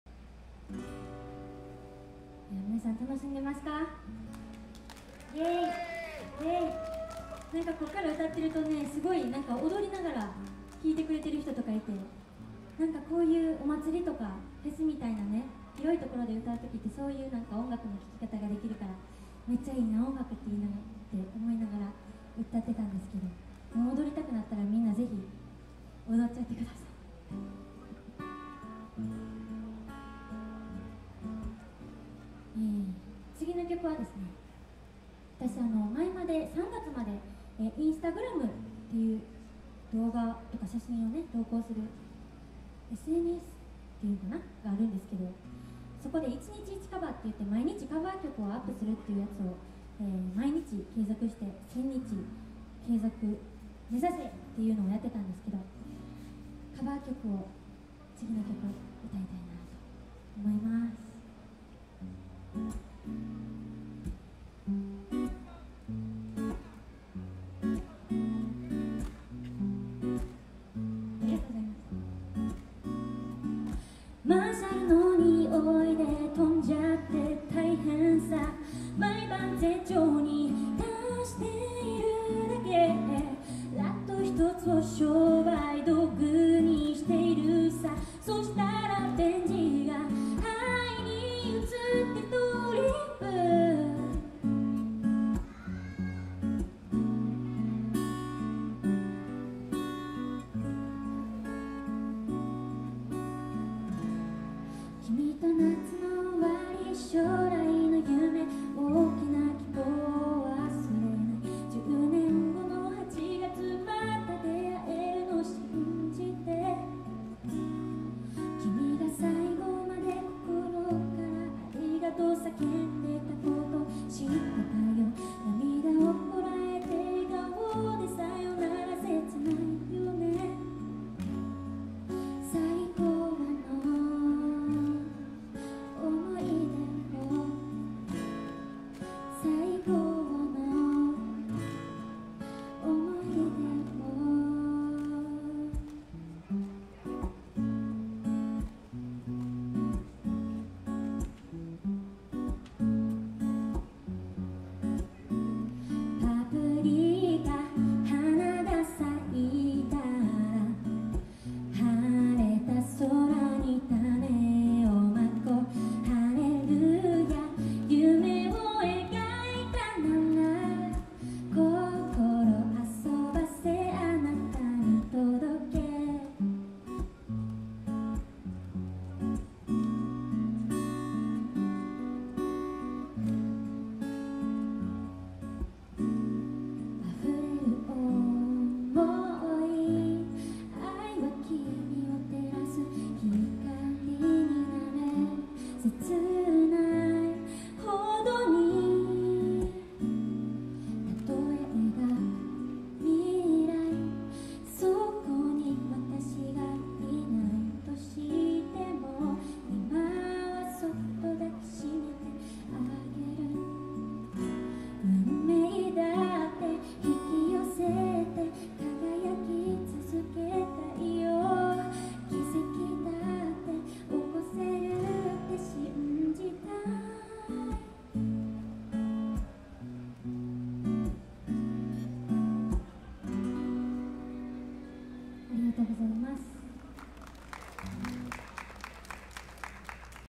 由利華『カバーメドレー』@ 神戸みなとまつり2019 in メリケンパーク 2019/07/14
由利華